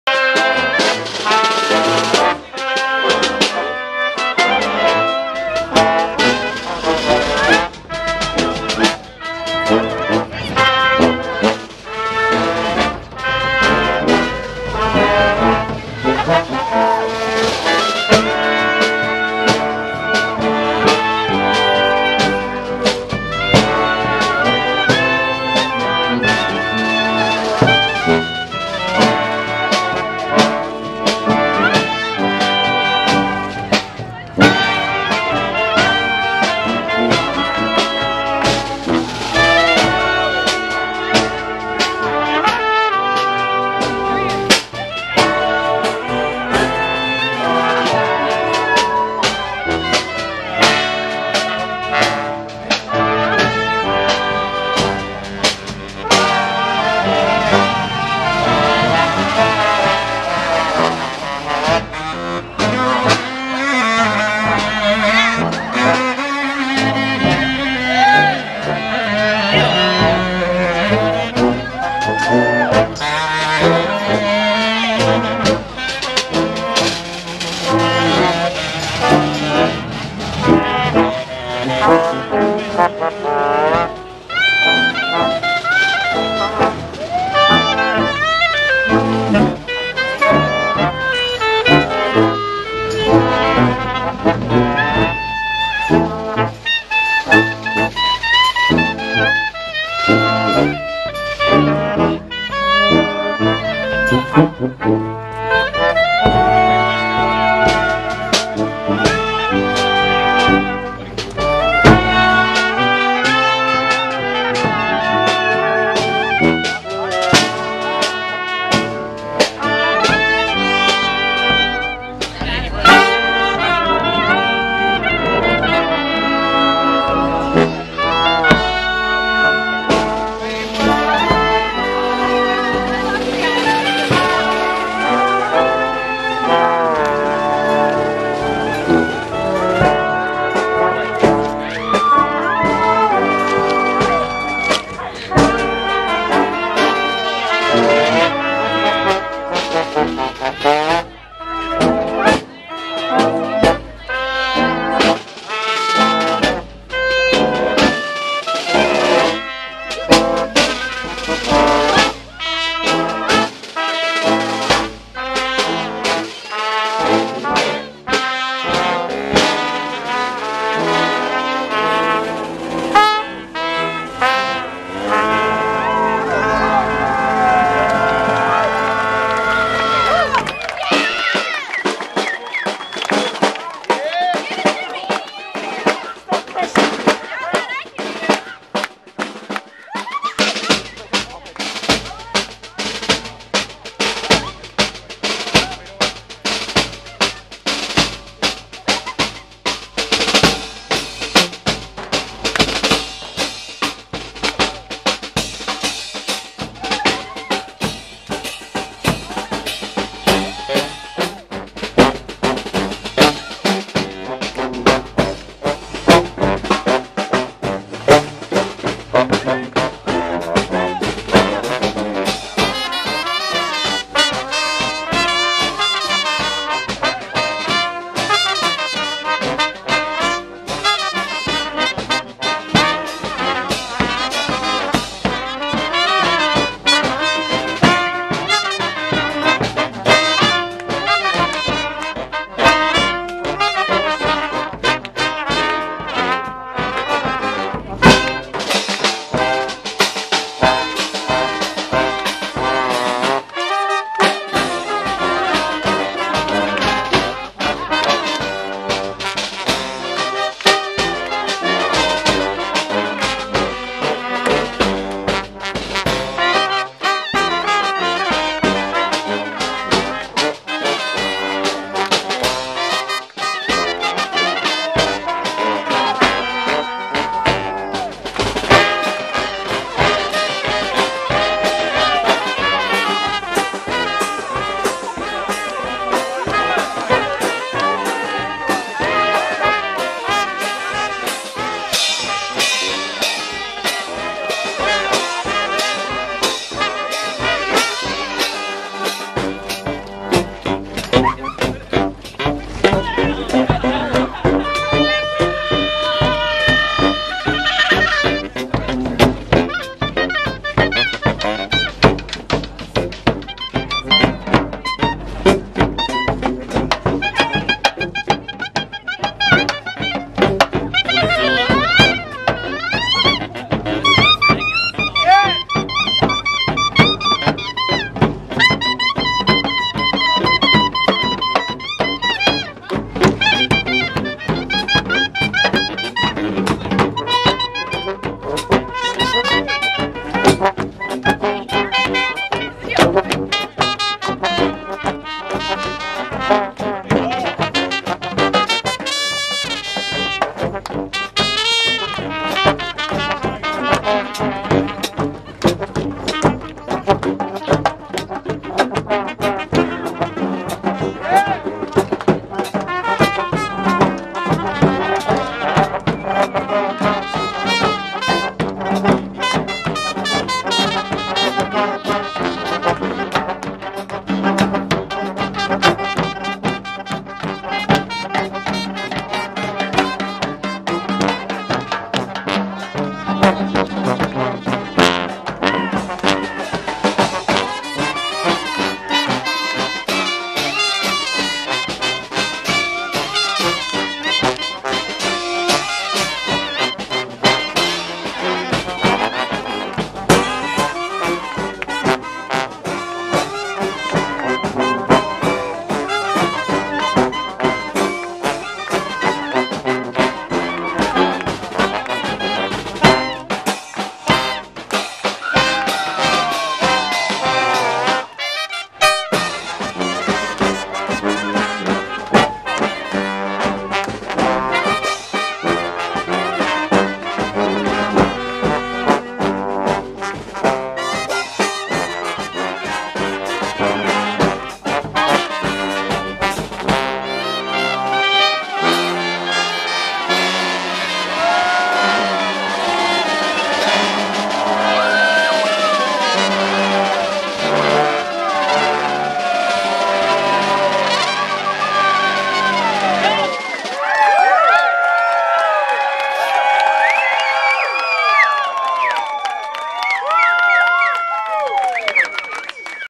Orkestar Zirconium at Official Bad Art Museum of Art and Cafe Racer
The most fabulous plays at Cafe Racer and the fun spills out into the street